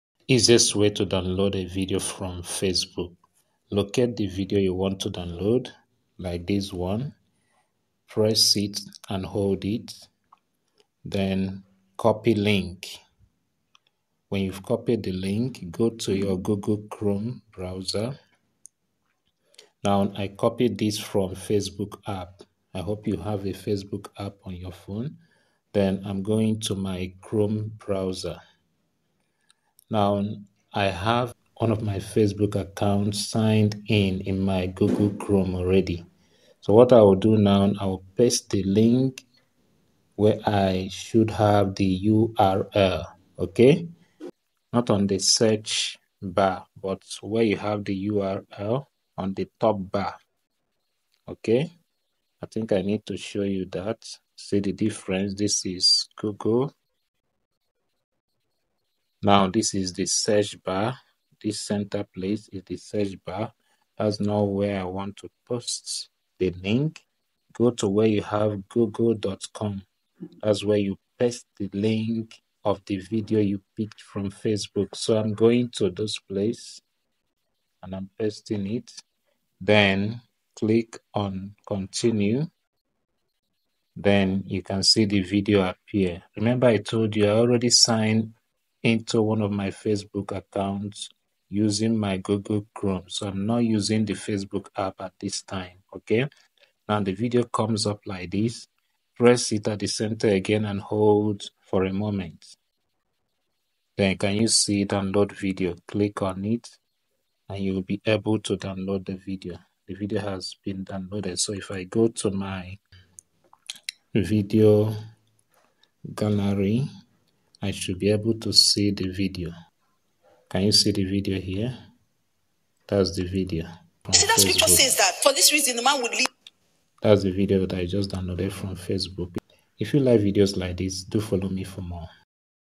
Easiest Way to Download Facebook Videos Using Google Chrome Browser
This is about the Easiest Way I use to download Videos from Facebook using Google Chrome Browser